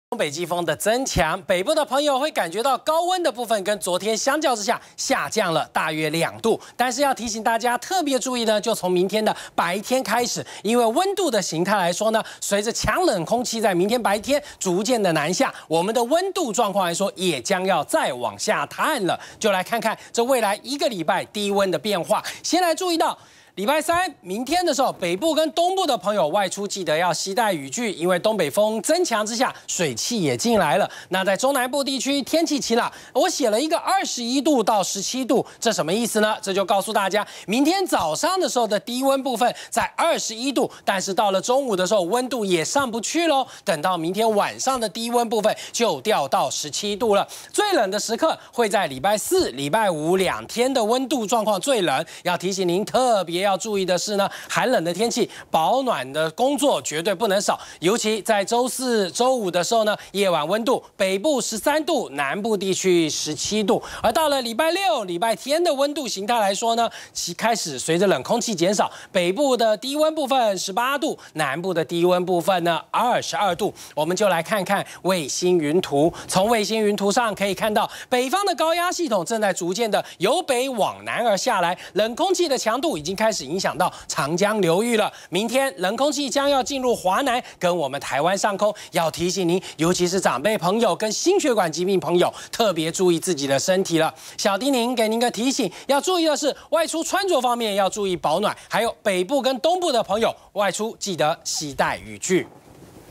20151124中天新聞 【氣象】週四冷氣團影響 北部低溫探13度
北方的高壓系統正在逐漸由北往南移動，明天開始冷氣團的冷空氣會開始慢慢接近，白天起溫度下降。